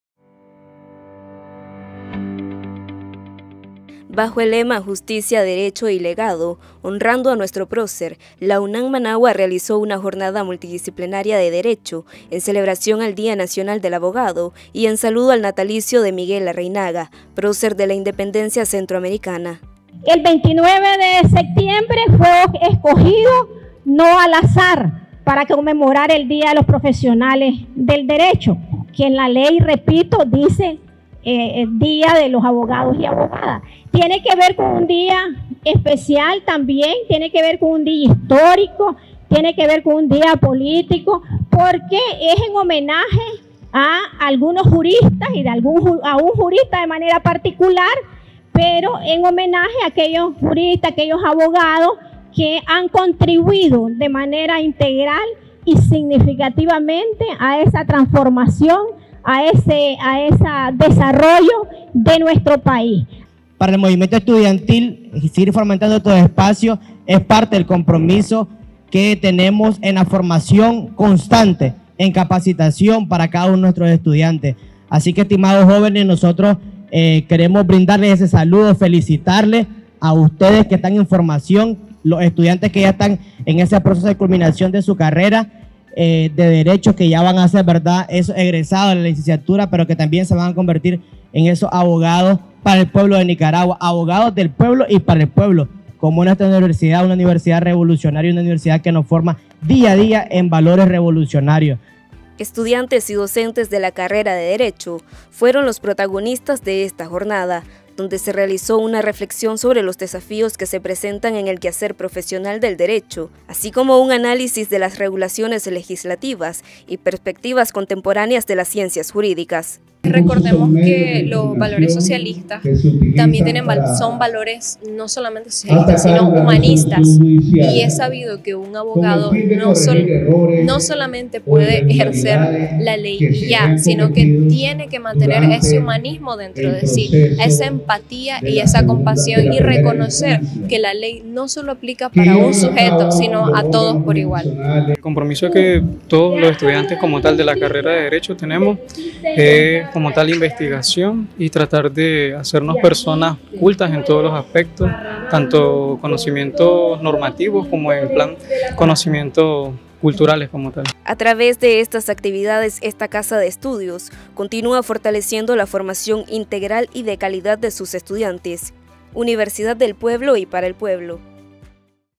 Jornada Multidisciplinaria Perspectivas Contemporáneas de las Ciencias Jurídicas
Futuros profesionales del Derecho participan en Jornada Multidisciplinaria Perspectivas Contemporáneas de las Ciencias Jurídicas, desarrollada bajo el lema: «Justicia, Derecho y Legado: Honrando a Nuestro Prócer».